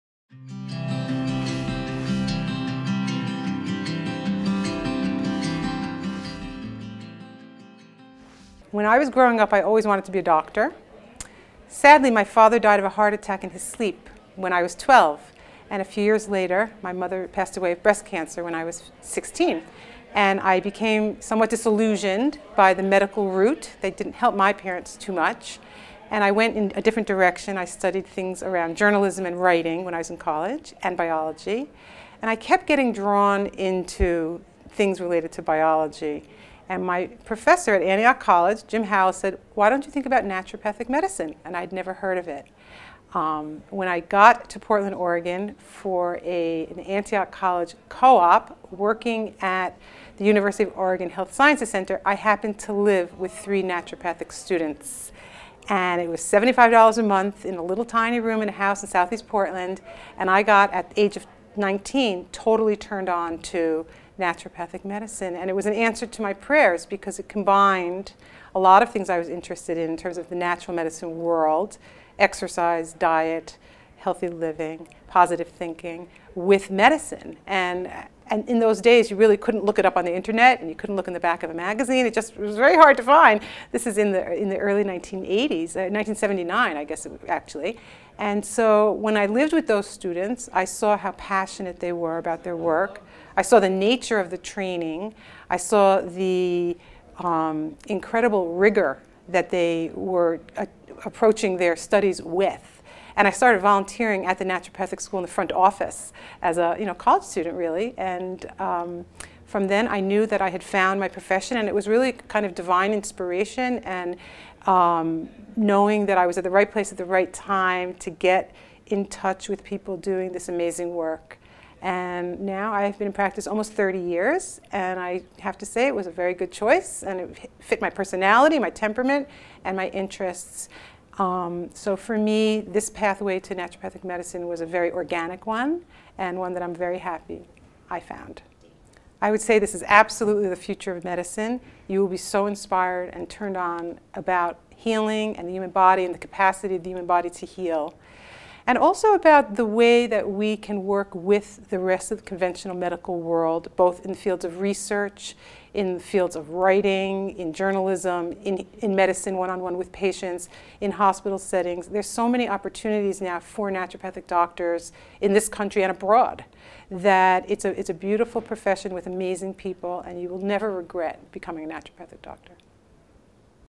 A Journey into Naturopathic Medicine - Amy Rothenberg
AANMC Career Pathways Project - Hear from Amy Rothenberg about her journey into naturopathic medicine.